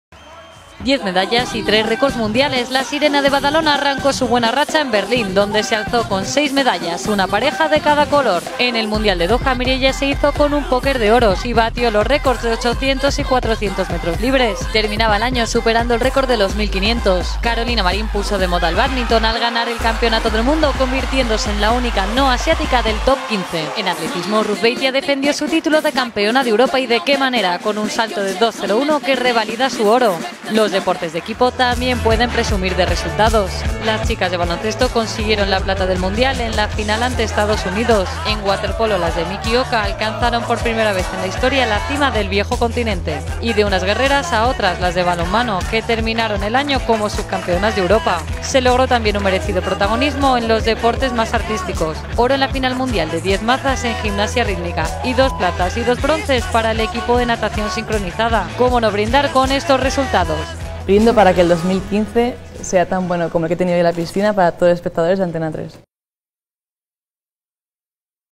Guerreras y reinas, las mujeres mandaron en 2014
Mucha guerra es la que han dado nuestras chicas de balonmano, baloncesto y waterpolo en estos últimos doce meses, triunfando en Mundiales y Europeos.